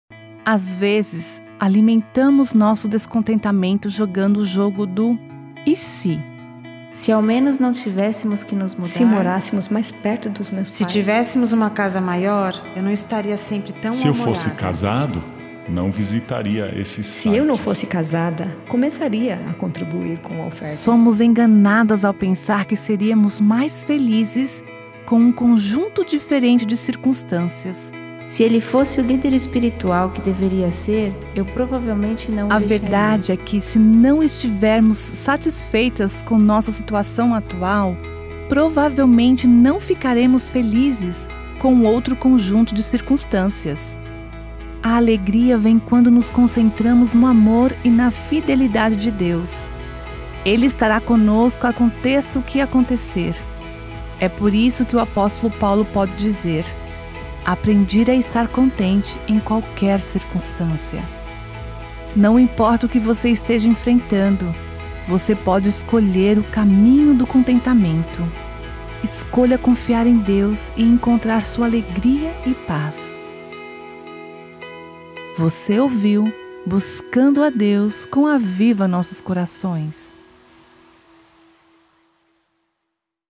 Perguntando "e se?"
Nancy: Às vezes, alimentamos nosso descontentamento jogando o jogo do ""E se?""
Mulher 1: Se ao menos não tivéssemos que nós nos mudar...
Mulher 2: Se morássemos mais perto dos meus pais...
Muher 3: Se tivéssemos uma casa maior não estaria sempre tão mal-humorada.
Homem: Se fosse casado, não visitaria esses sites.
Mulher 4: Se eu não fosse casada, começaria a contribuir com ofertas.
Nancy: Somos enganadas ao pensar que seríamos mais felizes com um conjunto diferente de circunstâncias.
Mulher 5: Se ele fosse o líder espiritual que deveria ser, eu provavelmente não o deixaria.
Nancy: A verdade é que se não estivermos satisfeitas com nossa situação atual, provavelmente não ficaremos felizes com outro conjunto de circunstâncias.
A alegria vem quando nos concentramos no amor e na fidelidade de Deus. Ele estará conosco aconteça o que acontecer. É por isso que o apóstolo Paulo pode dizer: ""Aprendi a estar contente em qualquer circunstância"".
Não importa o que você esteja enfrentando, você pode escolher o caminho do contentamento. escolha confiar em Deus e encontrar sua alegria e paz